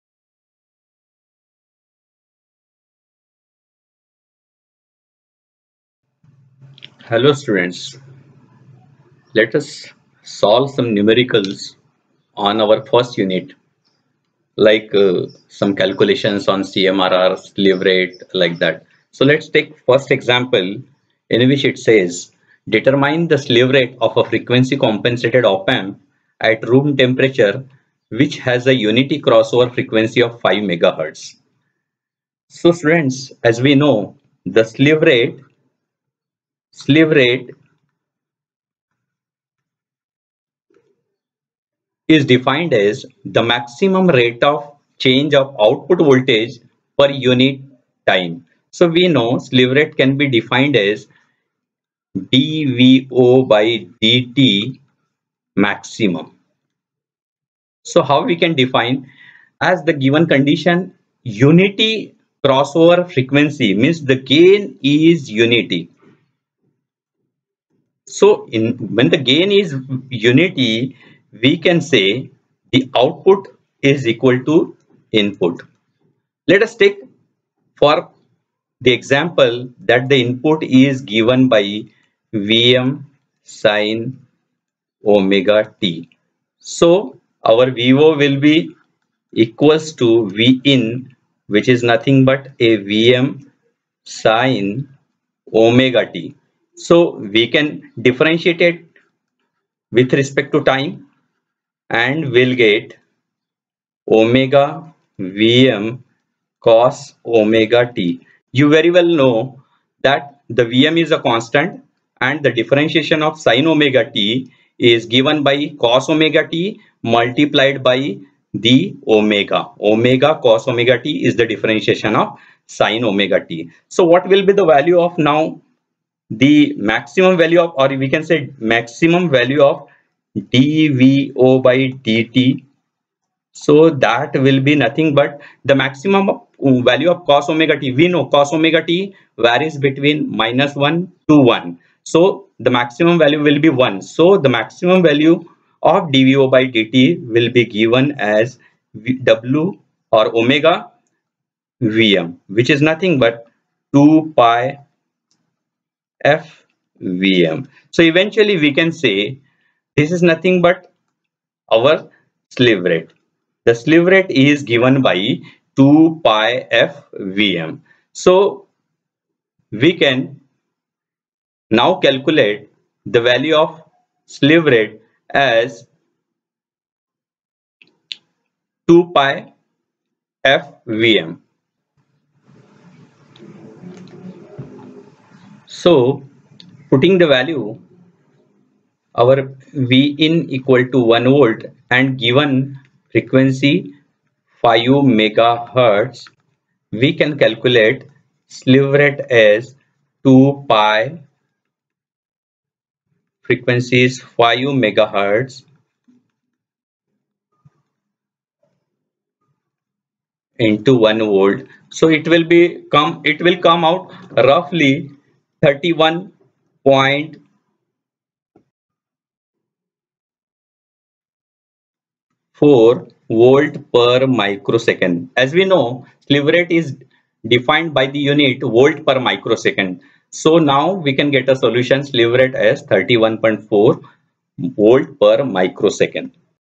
Numerical | Slew Rate | op amp | V H Mankar
This video is on numerical on Slew rate associated with op amp in linear integrated circuits course. This is helpful for undergraduate and diploma students in electronics.